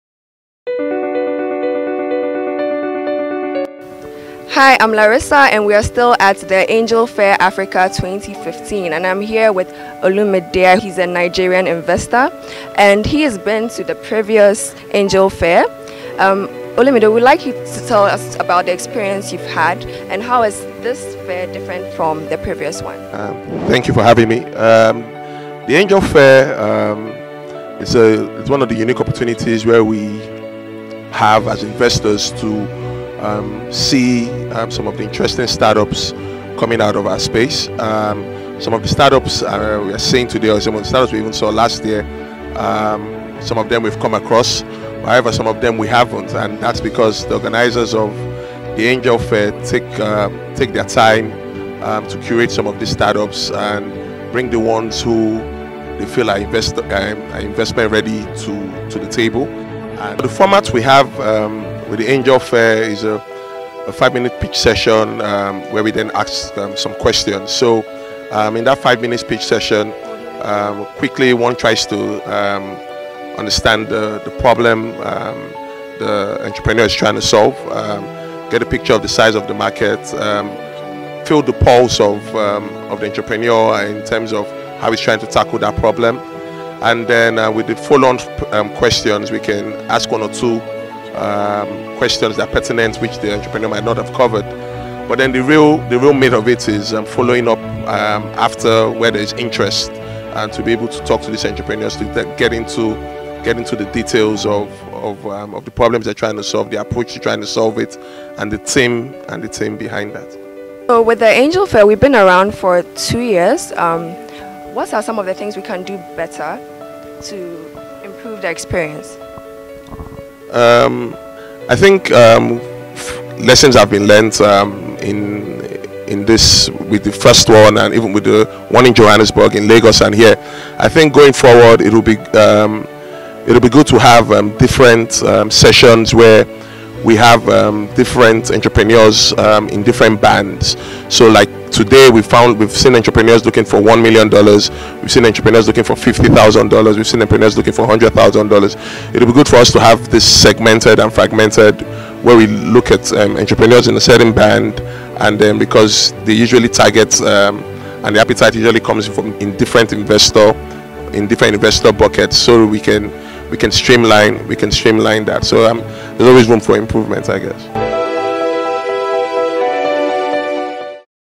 Angel Fair Africa 2015 - Investor's Feedback - Olumide Soyombo of Leadpath
Olumide Soyombo of Leadpath suggests that, the best way to help raise funds for startups is to segregate both the Investors and the startups so those seeking to raise big money would meet those willing to invest big money and vice versa. In this video, Larisa Bowen-Dodoo interviews Olumide.